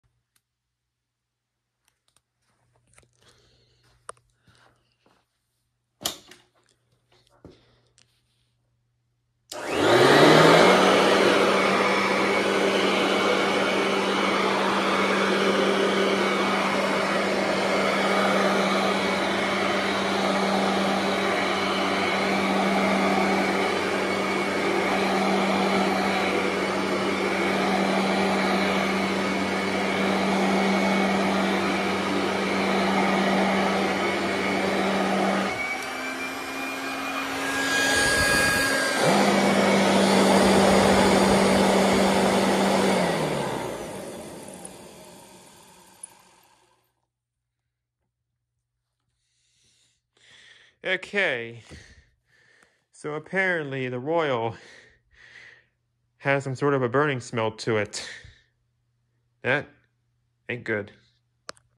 Royal Everlast at Colonial Baptist Church on 10/18/2023
Tonight it had a rubbery smell as I began to turn it off. Don’t know what it was. Still the best vacuum in the church!